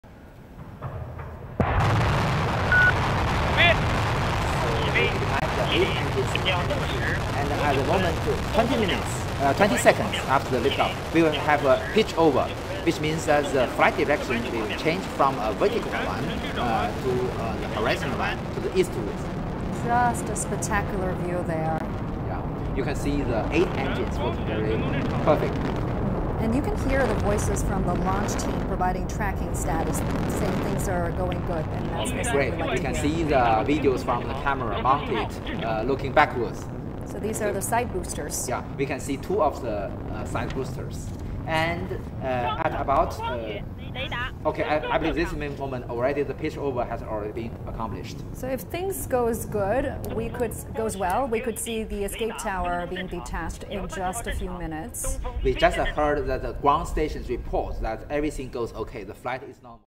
🚀 China lanzó la misión tripulada Shenzhou-18 hacia su Estación Espacial
China lanzó la nave espacial tripulada Shenzhou-18 con tres astronautas a su estación espacial desde el Centro de Lanzamiento de Satélites de Jiuquan en el noroeste de China, el jueves 25 de abril de 2.024 a las 8:59 pm hora local.
La nave de China sobre el cohete portador Gran Marcha-2F Y18 y con tres astronautas a bordo, surcó el cielo y entró en la órbita terrestre tras 10 minutos de su despegue, según lo confirmó la Agencia Espacial Tripulada de China (CMSA).
Luego de entrar en órbita, justo seis horas después del lanzamiento, la nave espacial Shenzhou-18 se acoplará al puerto de acoplamiento radial del módulo central Tianhe, formando así una nave nodriza de tres naves y tres módulos, según la CMSA.

Crédito: CCTV+ Space Station YouTube Channel